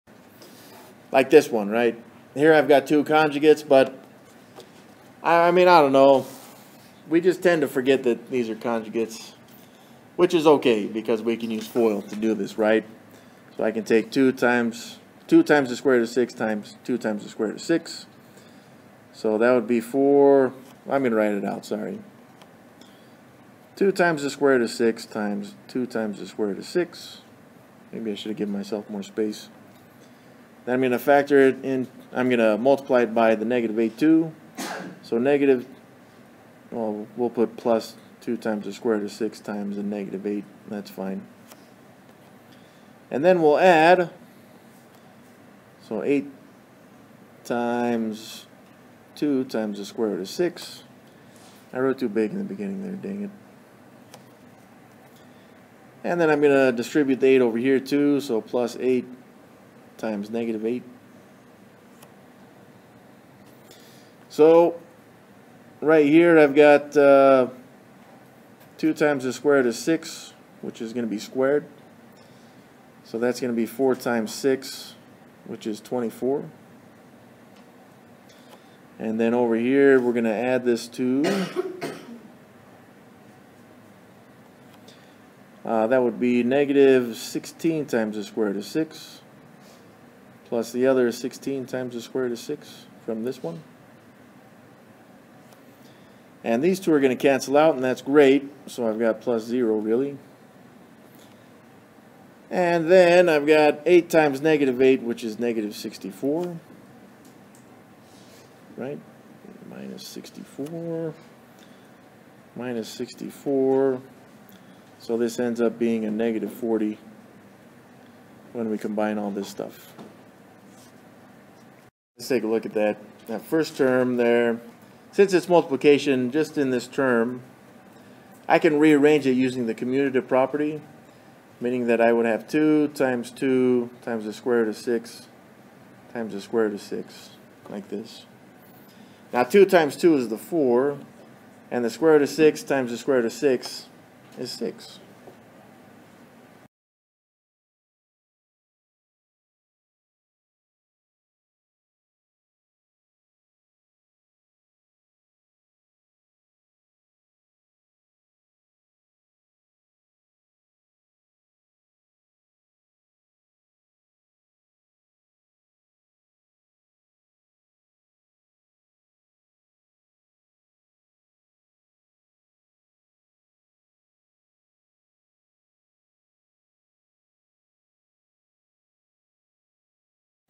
How to Multiply Radicals: Conjugates (a MATH 1010 Problem)
The problem:
Perform the indicated operation:
(2√6+8)(2√6−8)

The solution:
-40